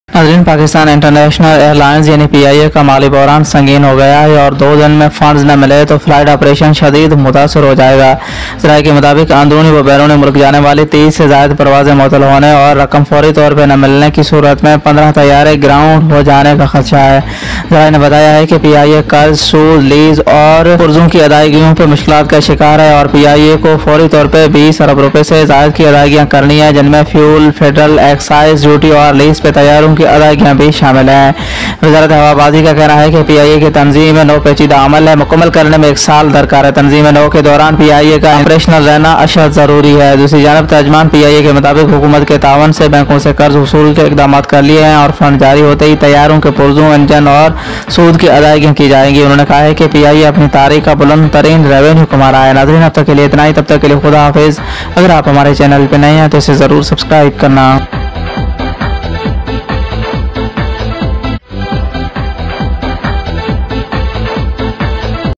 financial crisis of PIA is serious, flights are suspended and 15 planes are feared to be grounded
The financial crisis of PIA is serious, flights are suspended and 15 planes are feared to be grounded